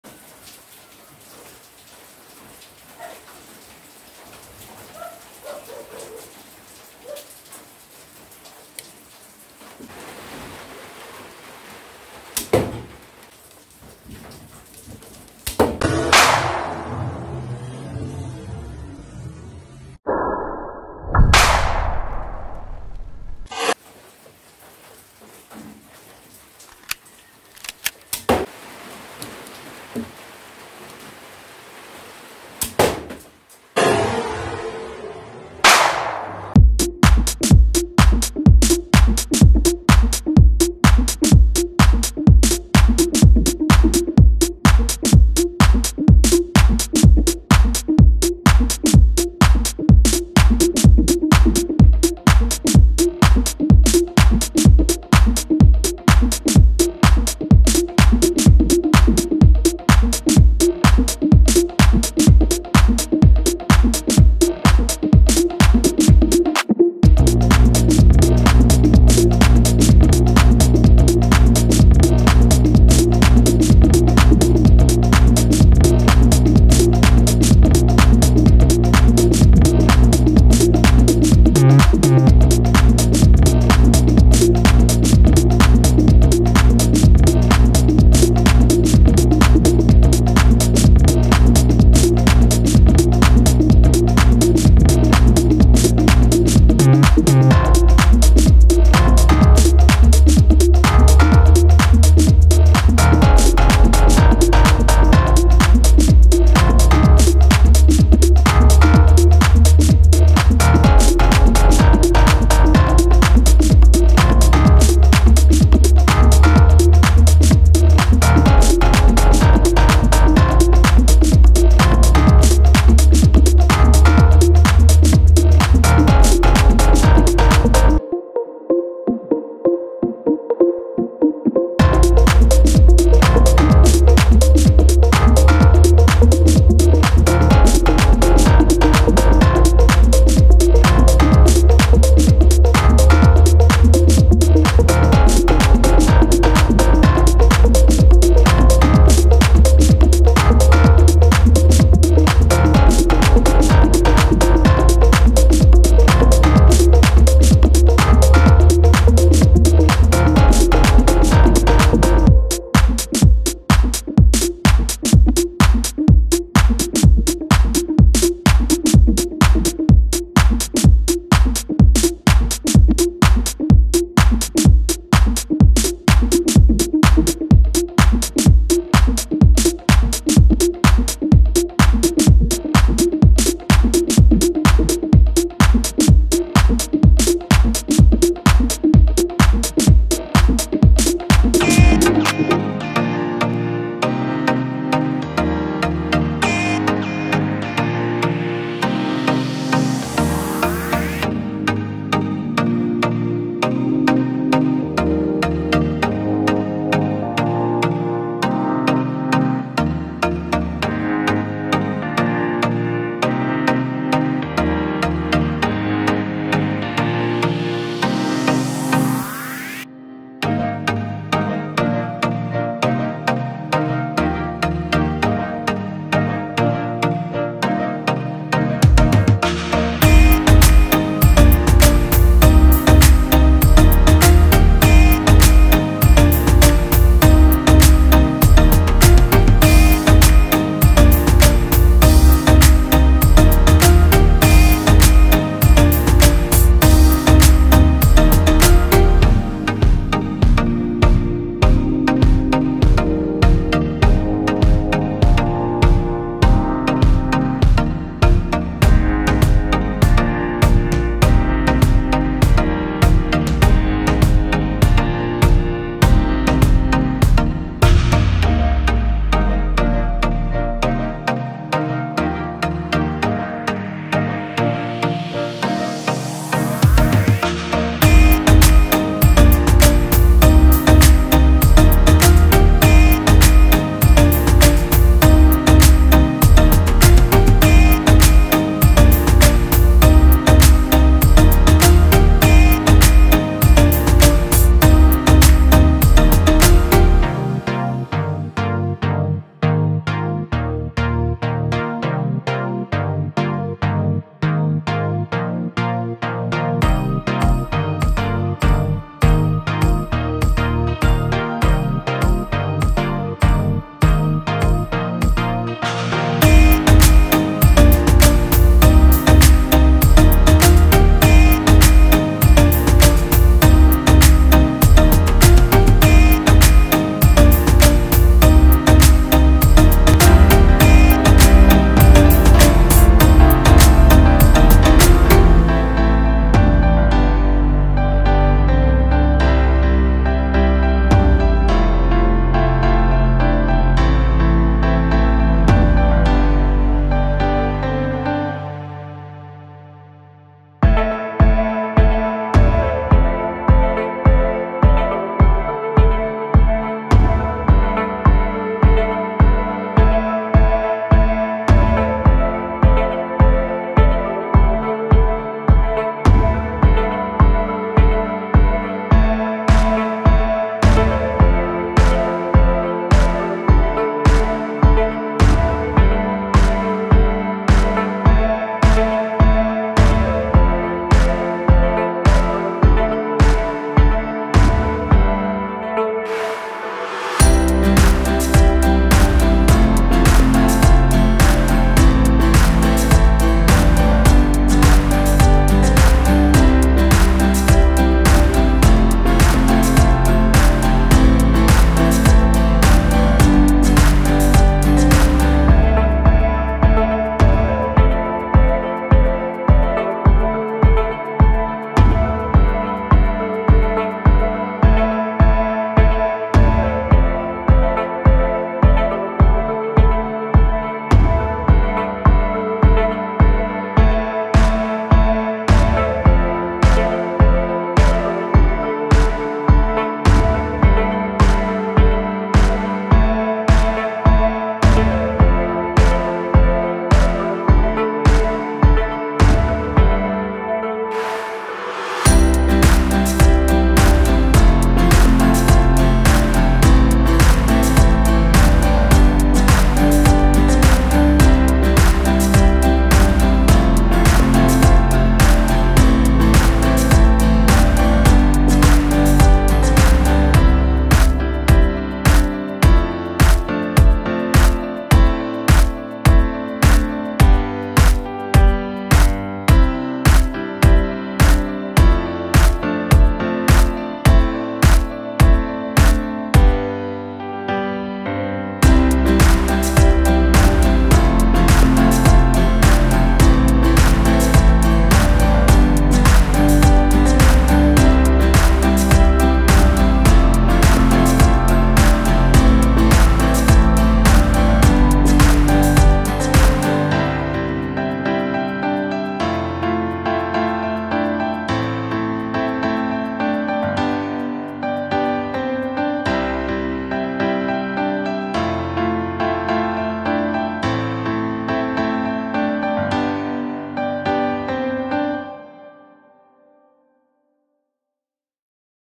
Diy Crossbow with Bamboo Arrow
Diy  Mini WOODEN CROSSBOW, with small Arrow , made of bamboo stick